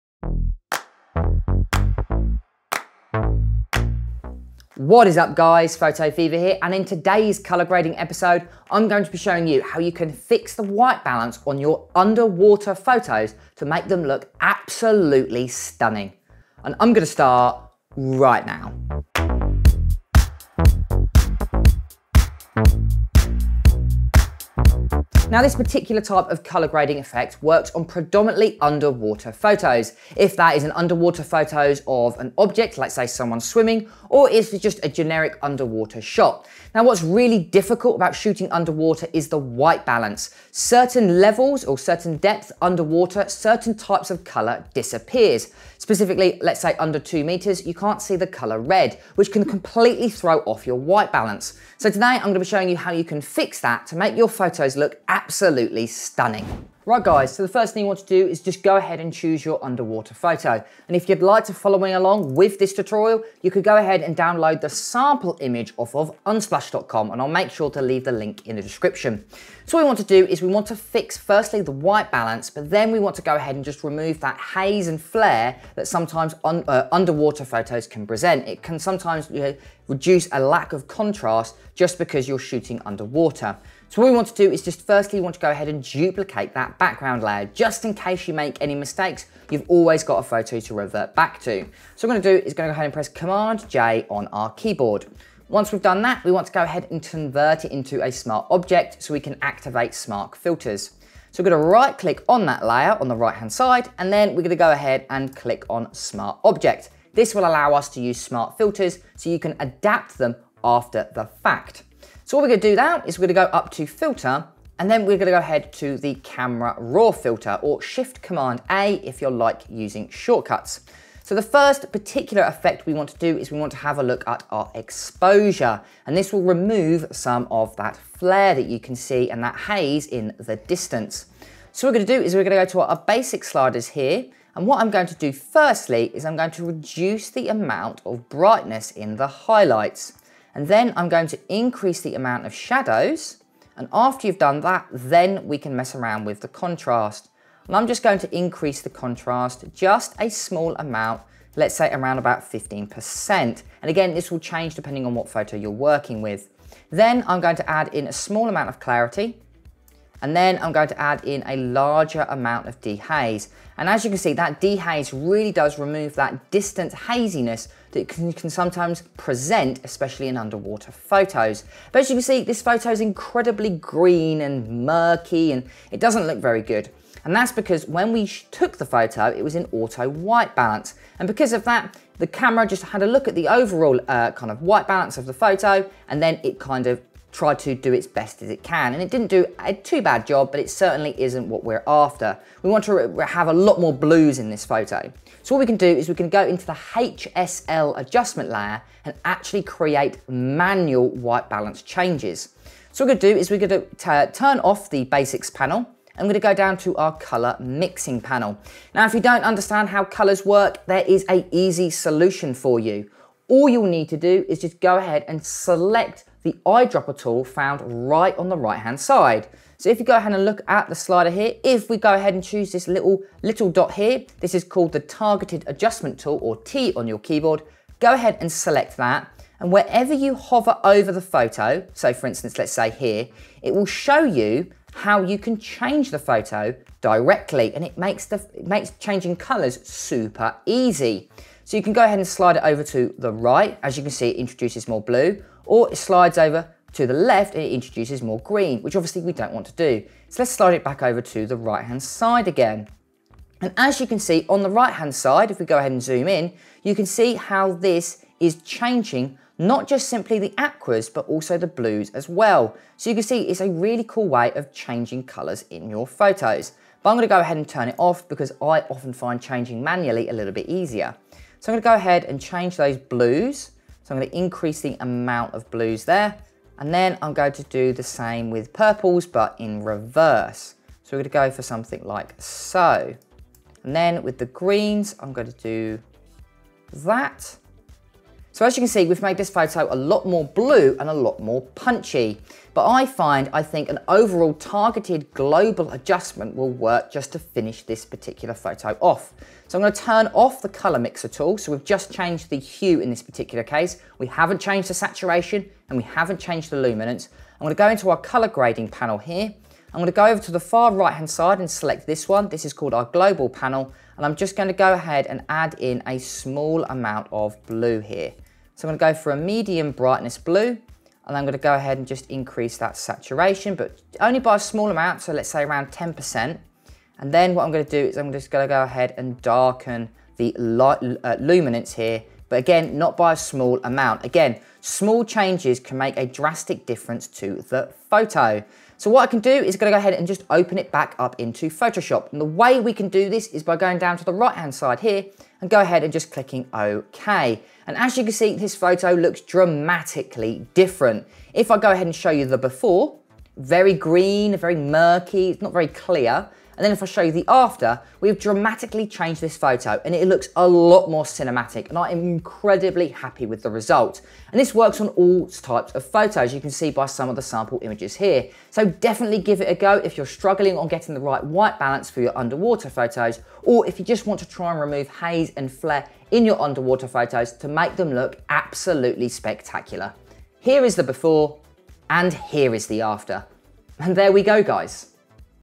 How to Edit Underwater Photos Like a PRO in Photoshop
In this colour grading episode I will show you how to edit underwater photos like a pro in Photoshop CC 2021. I will be using Photoshop and the Camera Raw Filter.

Also make sure to have a look at the pre-selected photo in the first link below to follow along. Credit to unsplash.com

🎥 How to Use The HSL COLOUR PANEL in photoshop 2021